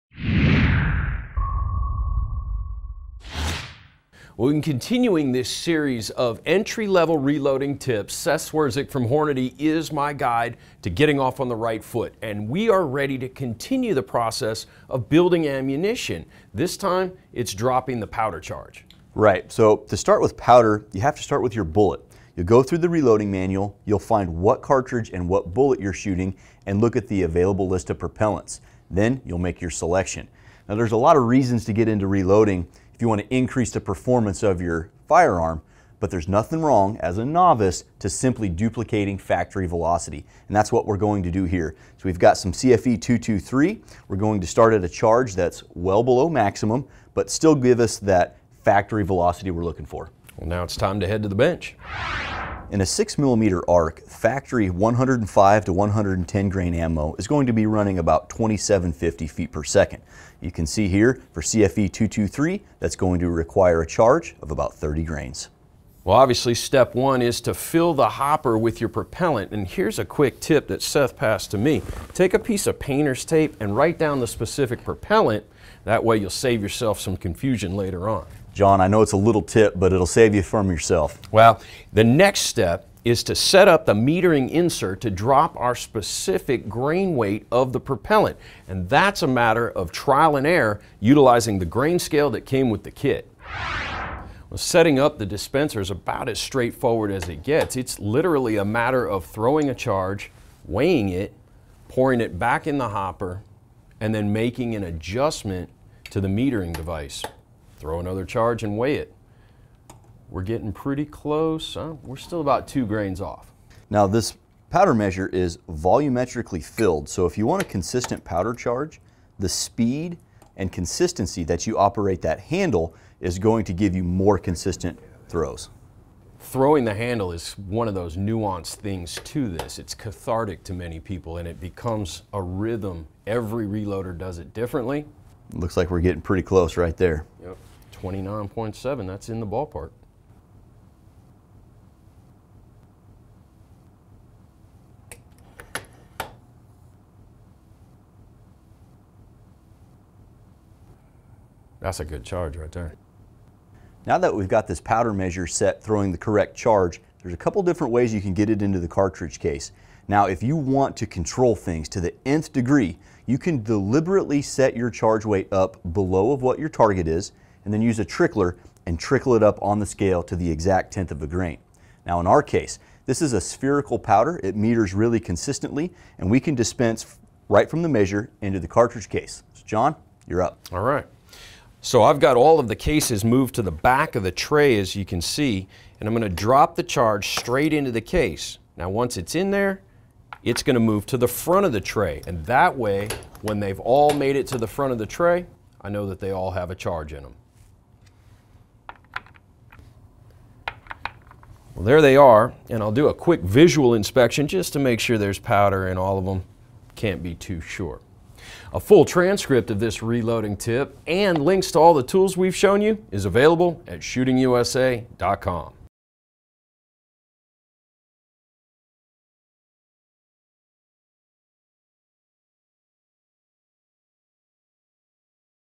Beginner Reloading Tips Part 3
Follow along with John Scoutten of Shooting USA as he is guided by our own Seth Swerczek in this five-part series of reloading tips. In this video, the guys detail powder selection and dropping accurate powder charges.